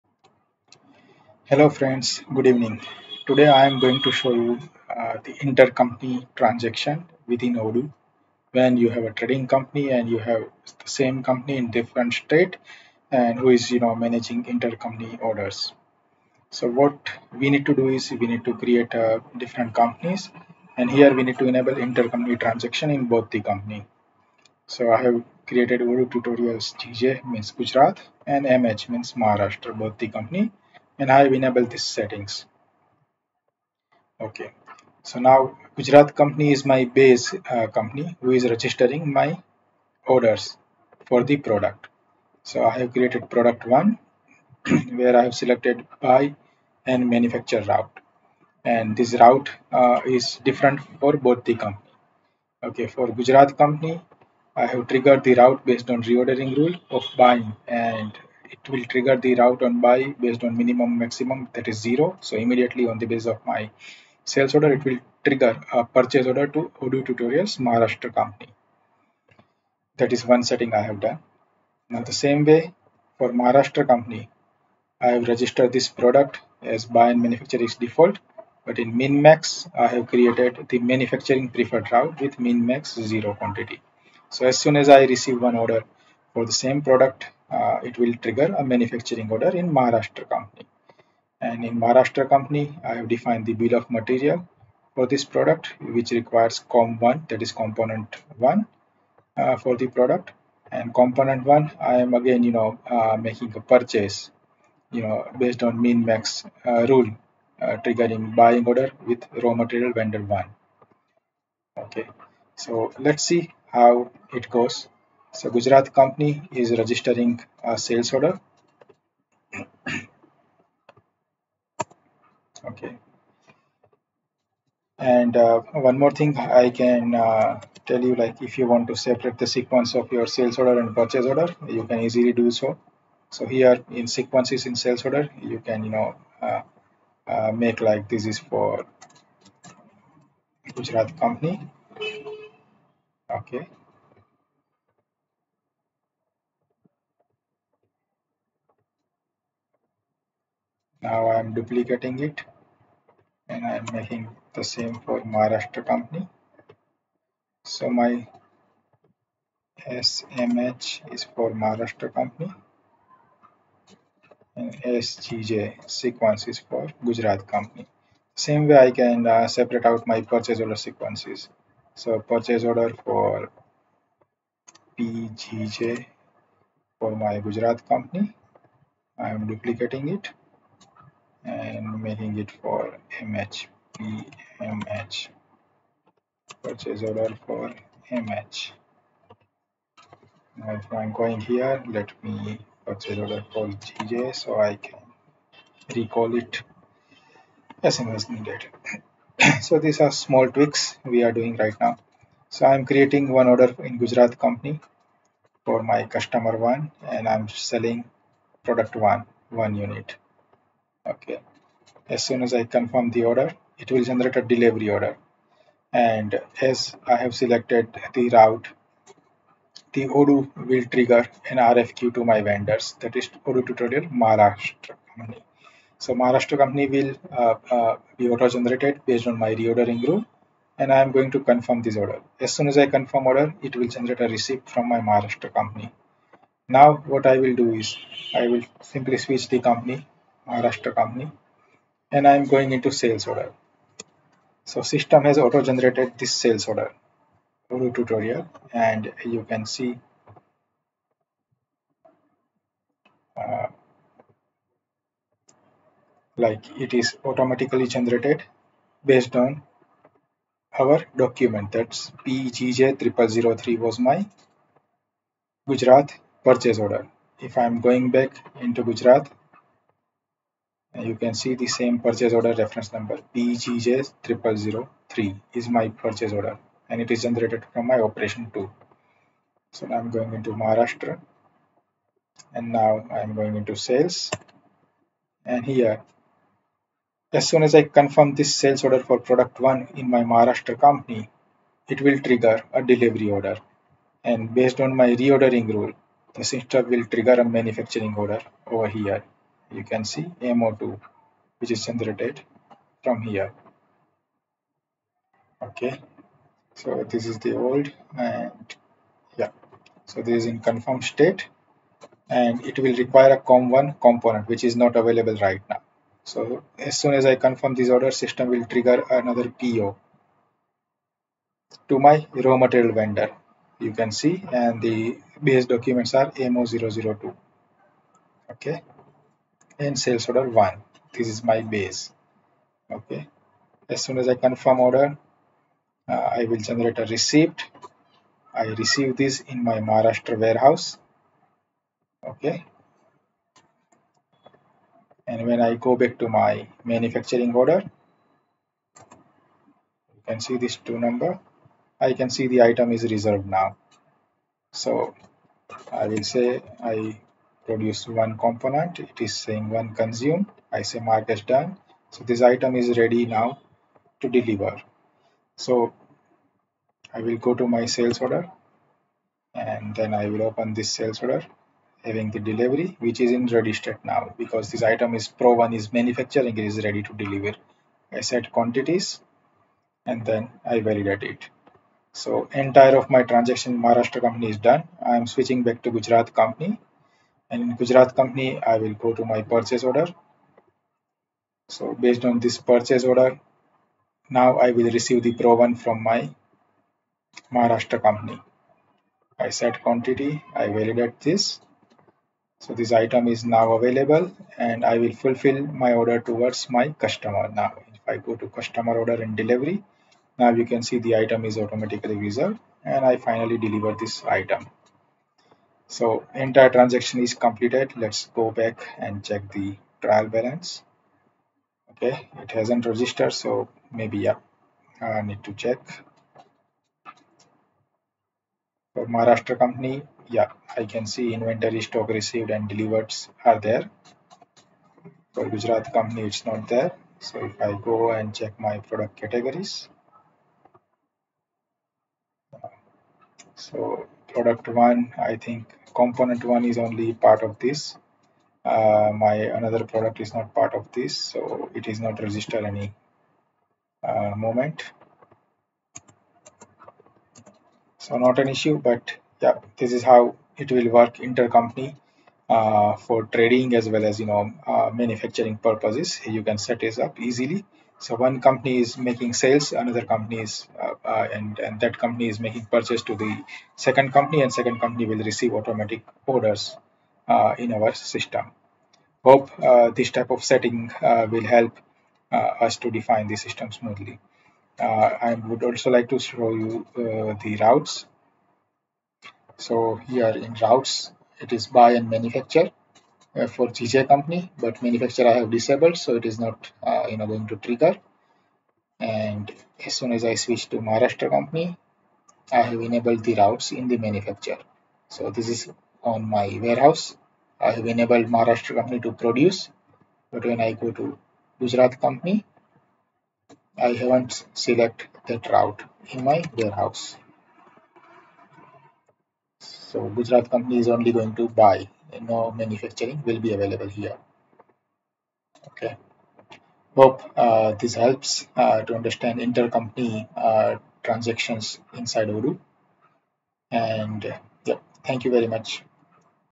Inter Company Transactions Odoo | Automate Sales Orders & Purchase Orders between two companies
Odoo has been offering a feature since long but consultants and developers are not sure how to configure and use inter company transaction features in Odoo. I have prepared a use case for two companies within India with same name from different states to make sale - purchase and sale - manufacture transactions smoothly in Odoo.

Please check this video to know how inter company transaction works to automate sales - purchase orders along with manufacturing orders based on reordering rules in Odoo.